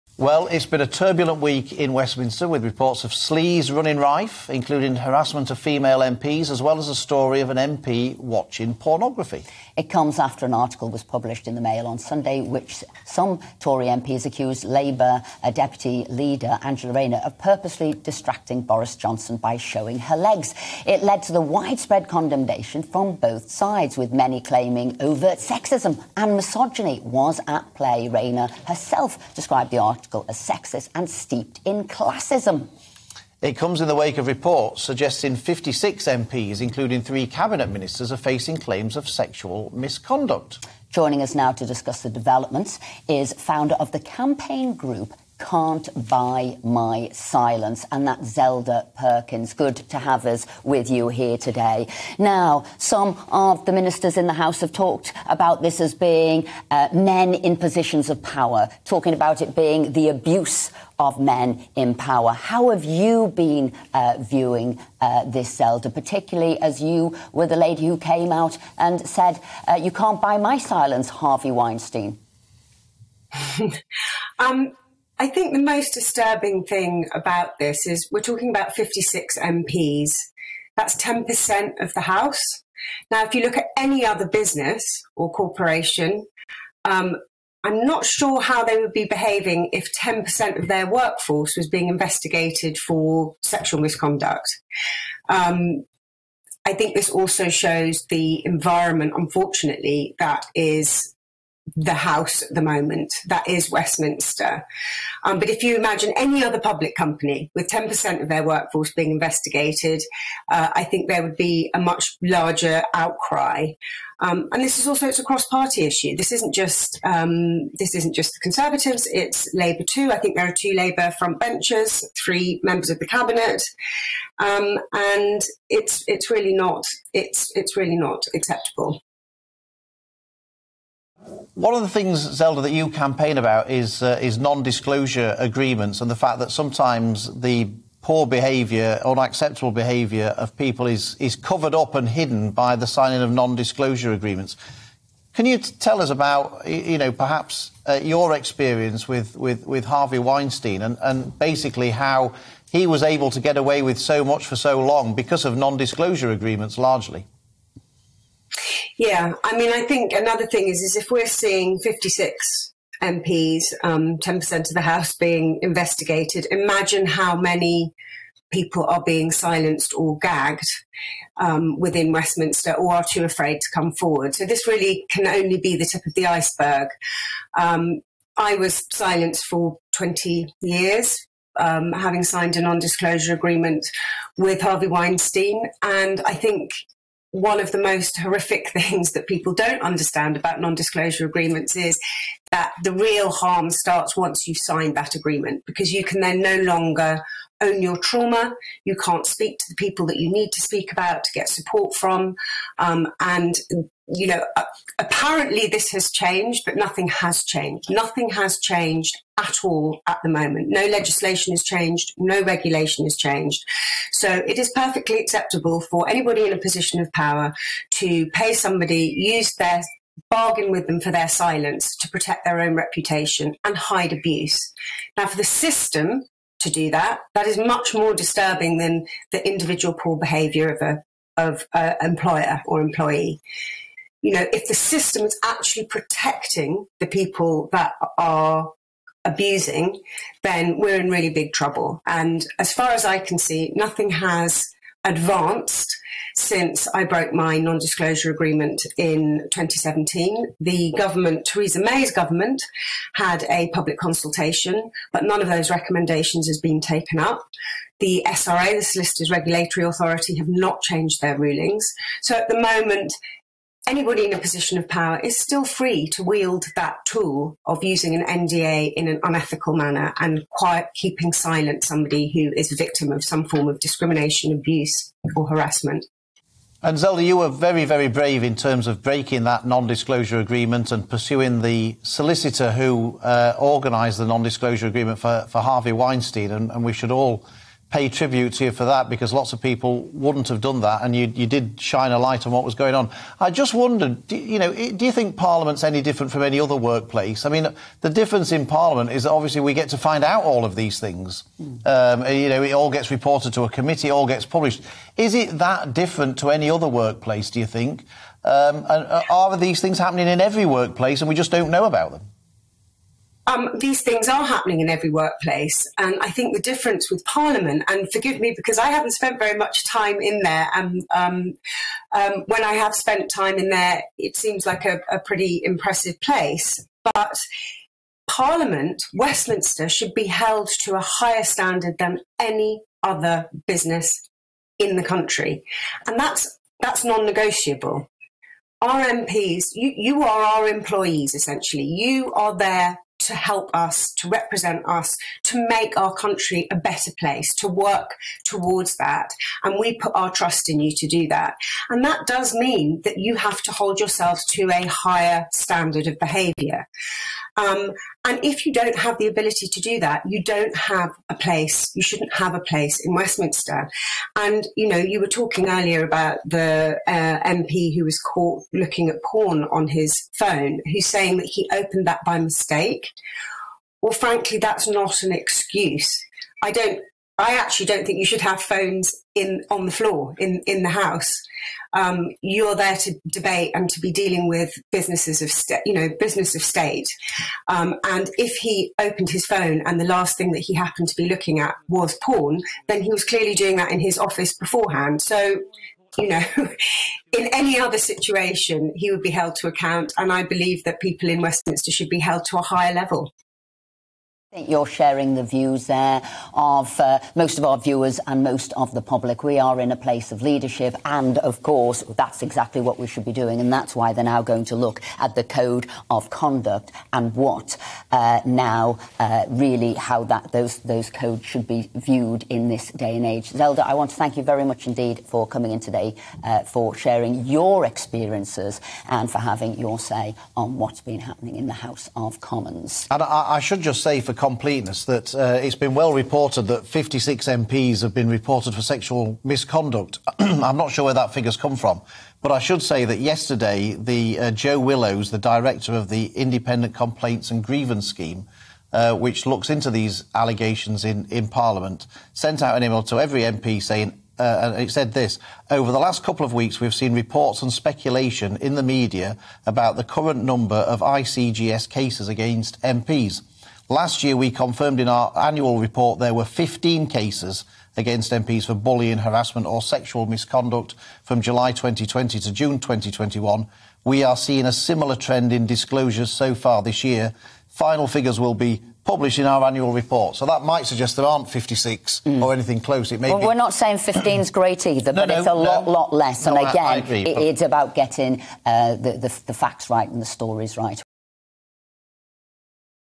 Zelda Perkins of Can't Buy My Silence discusses 56 MPs facing claims of sexual misconduct
'If you look at any other business or corporation, I'm not sure how they would be behaving if 10% of their workforce were being investigated for sexual misconduct.'

Co-founder of 'Can't Buy My Silence', Zelda Perkins, on 56 MPs facing claims of sexual misconduct.